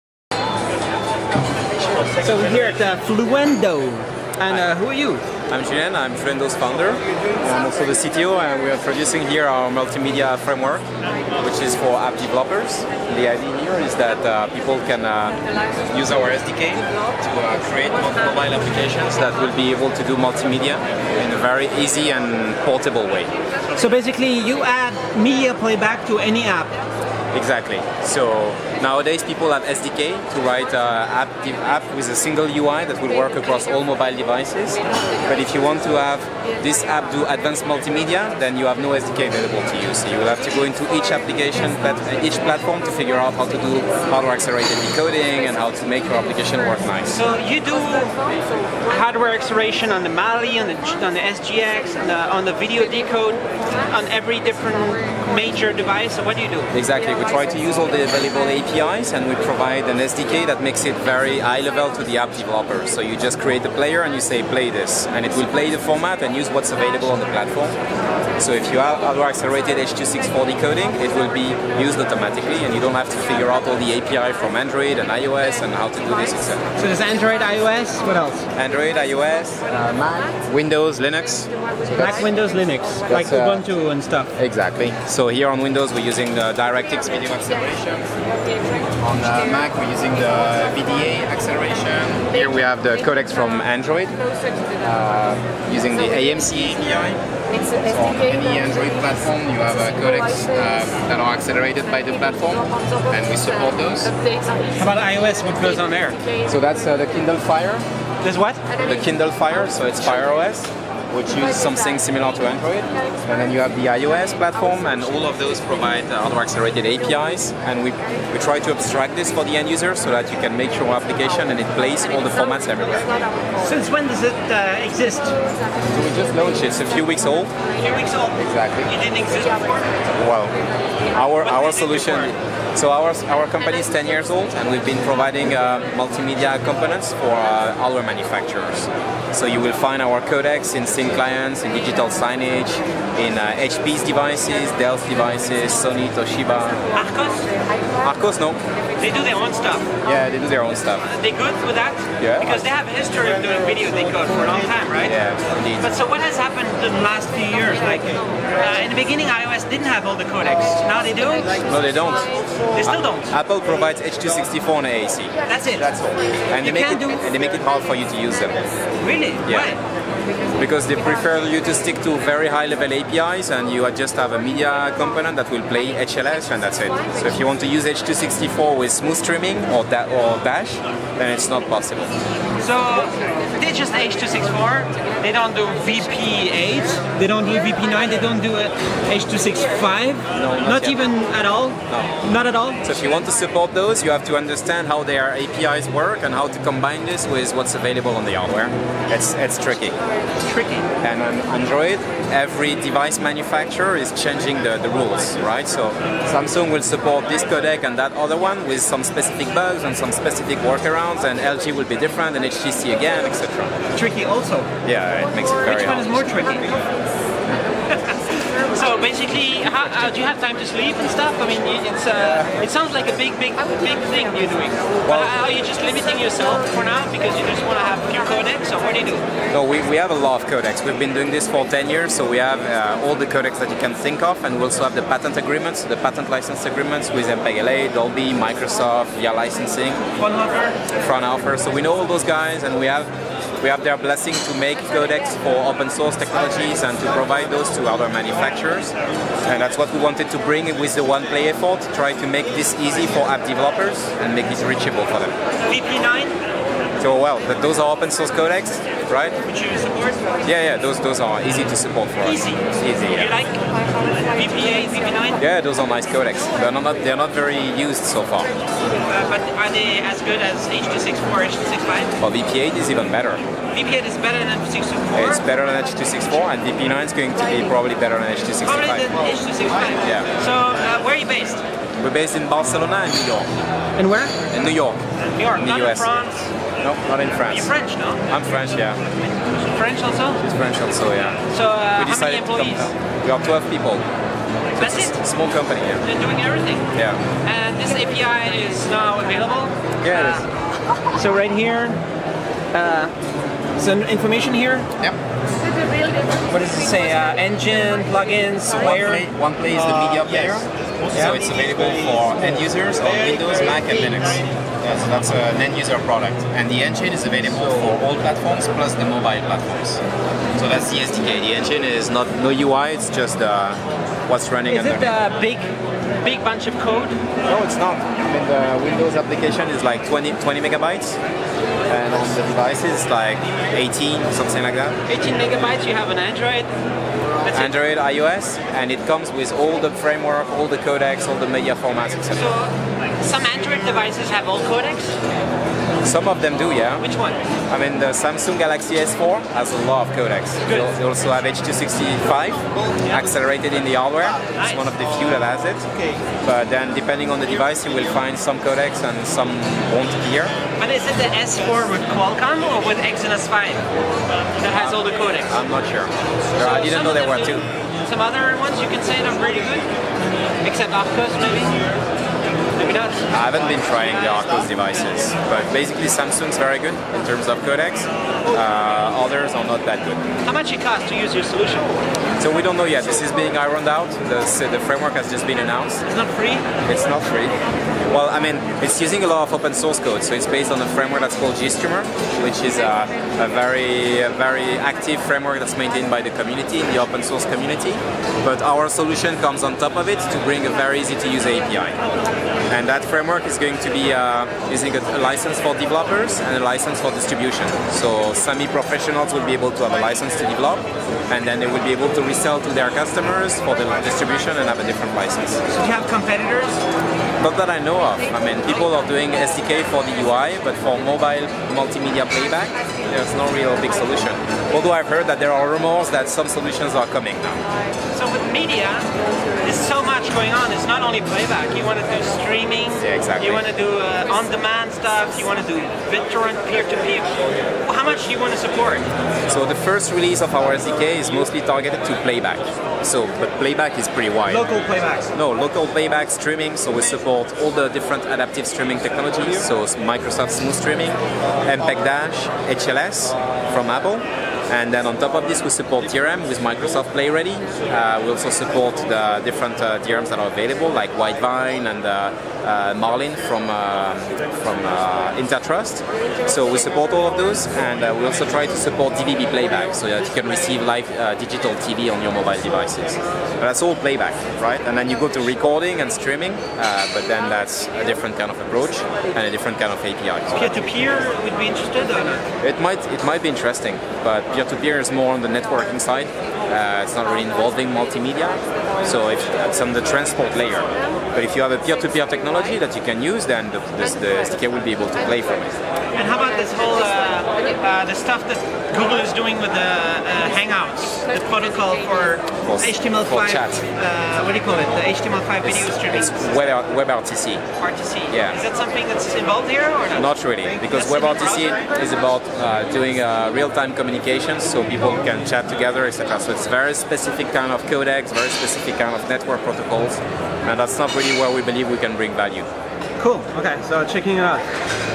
Fluendo Oneplay Cross Platform Multimedia
Fluendo is New York based provider of multimedia solutions and they have been in business for ten years. Fluendo oneplay can add multimedia support to any application on a variety of platforms. Fluendo oneplay is both an engine and an end user product. Fluendo oneplay enables you to use the same multimedia framework on a variety of platforms without using the different apis on each platform. Fluendo oneplay currently works on Android, iOS, Mac OS, Linux, and Windows. Fluendo oneplay is also hardware accelerated. Fluendo partners with large companies and can therefore use the technology and patents owned by those large companies. Fluendo's solution is available for both developers and distributors with an unannounced price.